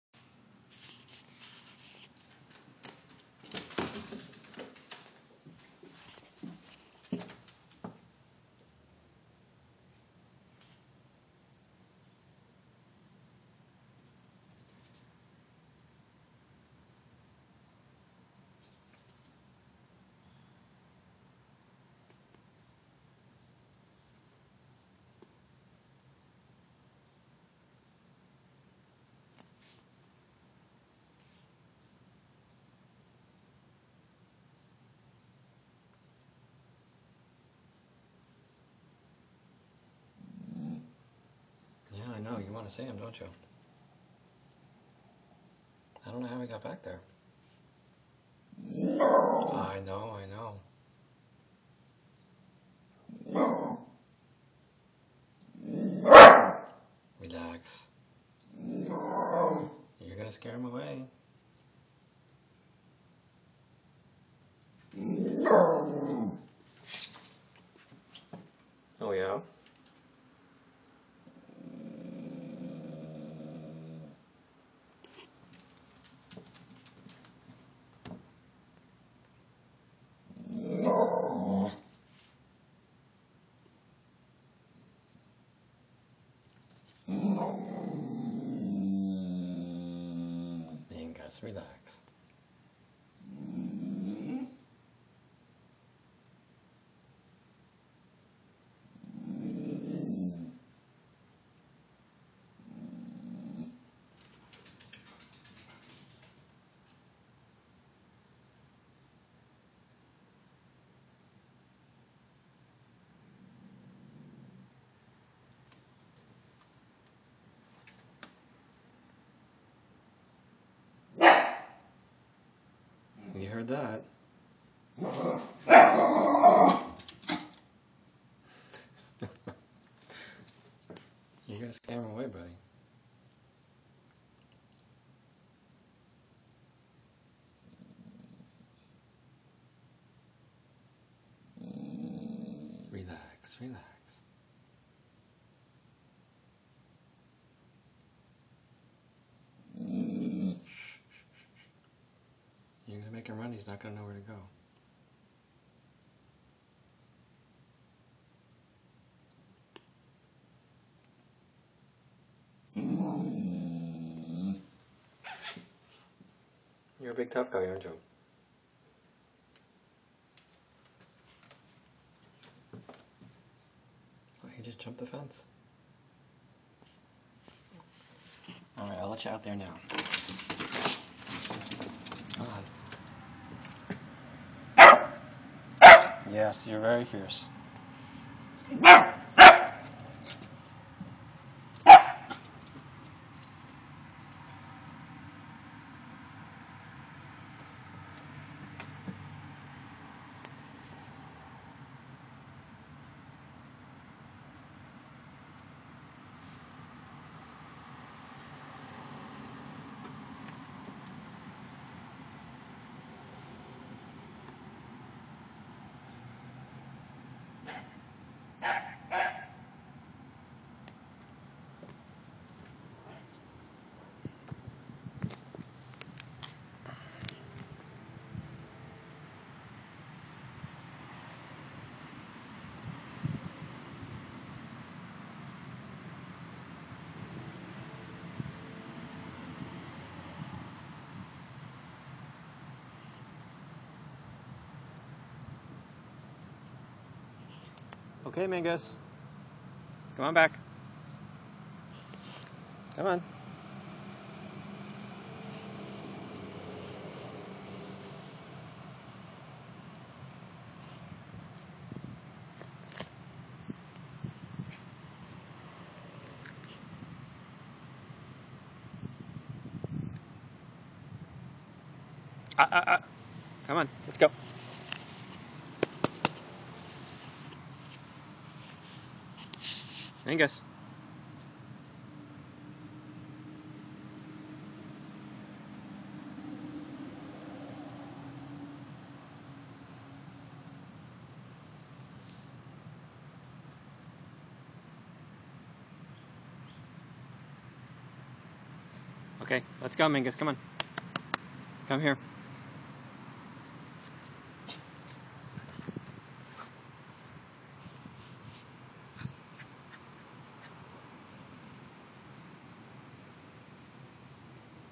Deer in the Backyard
Mingus alerted me to an intruder  in the backyard.